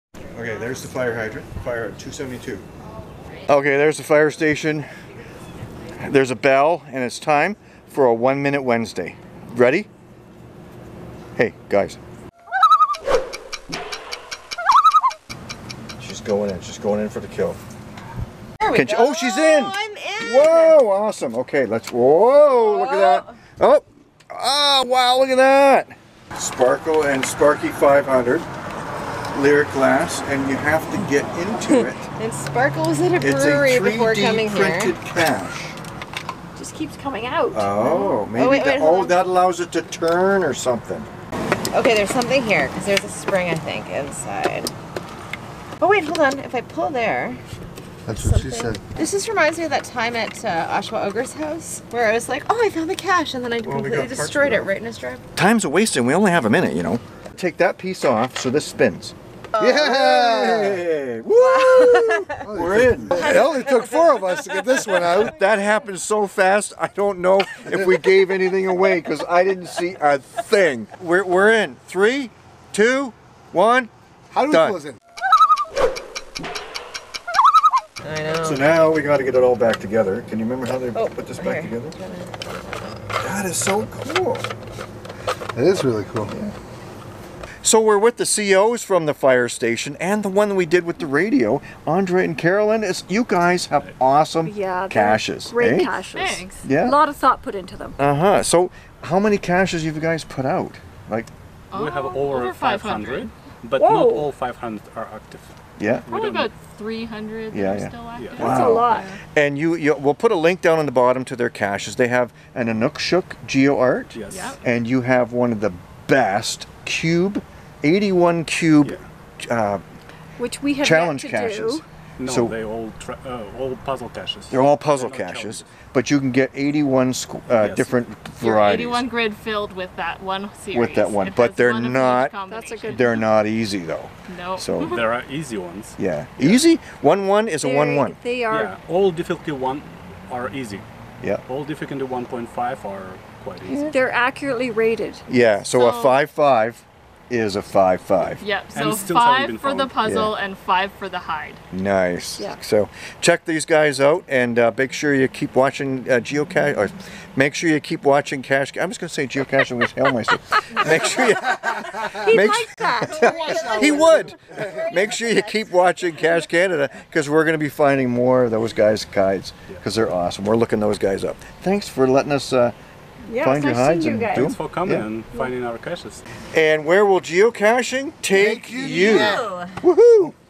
Fire Station Geocache Gadget Cache
Not just your run of the mill cache. Not just a cache at a fire station. It's also a Gadget Cache and we only have a minute to get in it. It's a One Minute Wednesday.